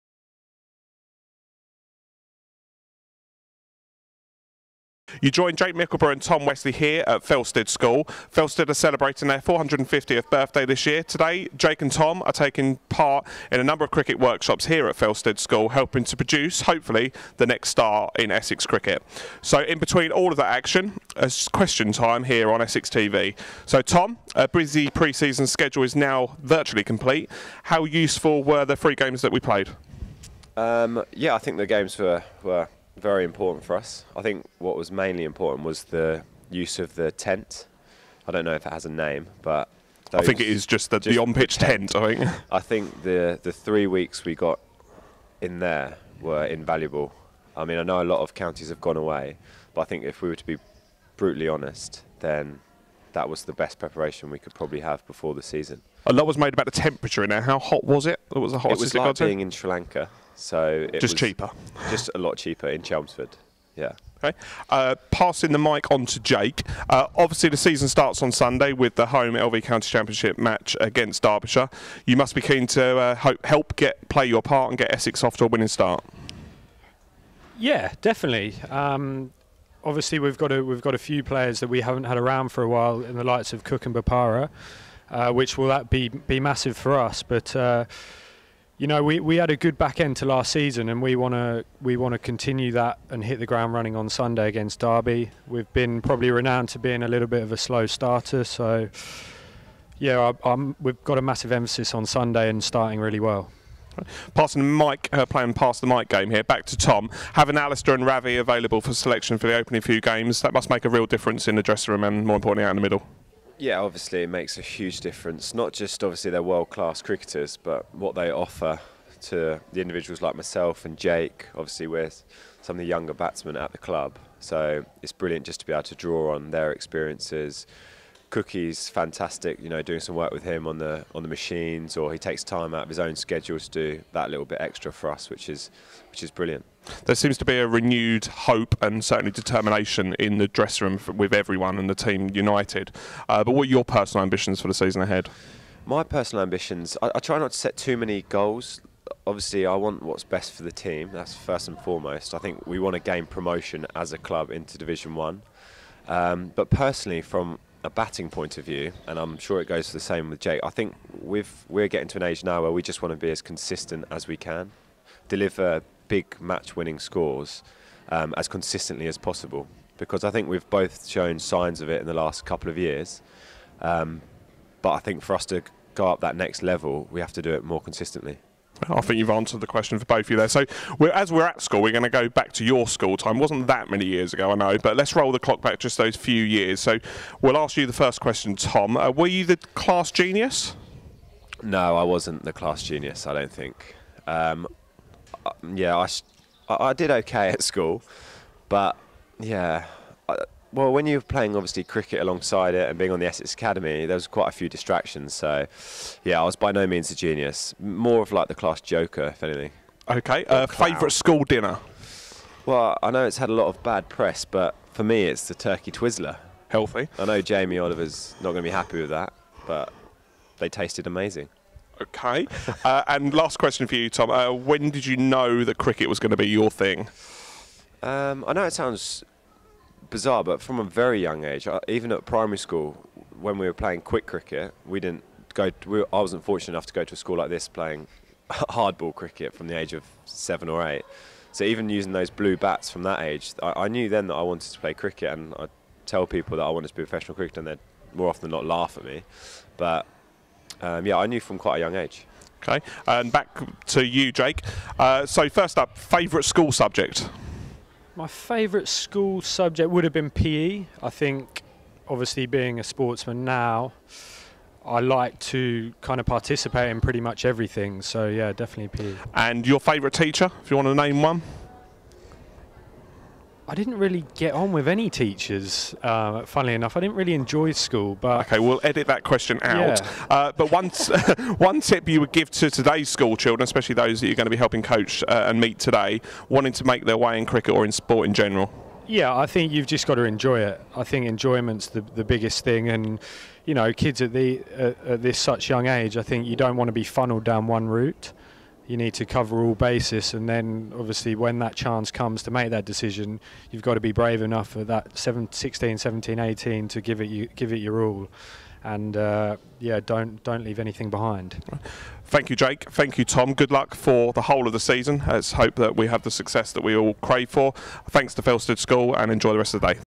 Tom Westley & Jaik Mickleburgh Gear Up for 2014
Tom Westley & Jaik Mickleburgh look ahead to opening game of the 2014 season after a successful pre-season